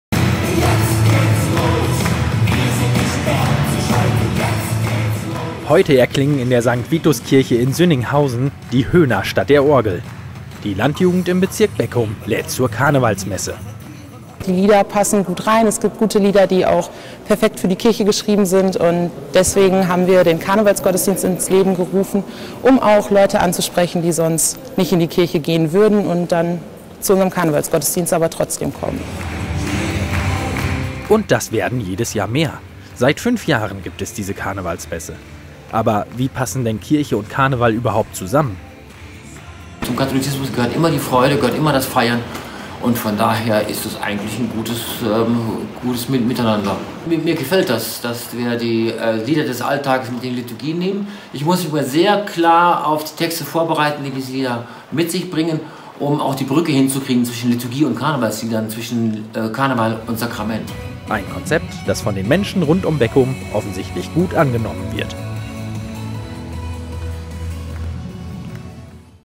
Karneval in der Kirche - Karnevalsmesse der Landjugend im Bezirk Beckum
Höhner statt Orgel hieß es in der Karnevalsmesse der Landjugend im Bezirk Beckum. Zum fünften Mal veranstalteten die Jugendlichen diesen besonderen Gottesdienst. Diesmal in der St.-Vitus-Kirche Sünninghausen.